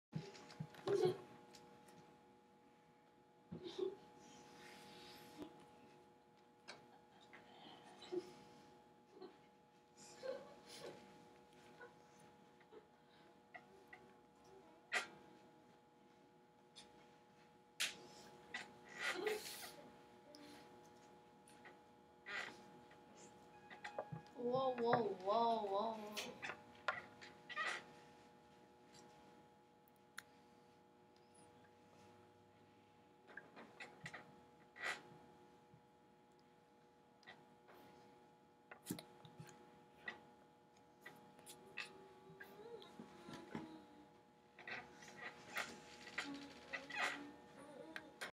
Chang May defines "incognito"
CSD summer school program. Stacy Eilbert's bilingual language arts class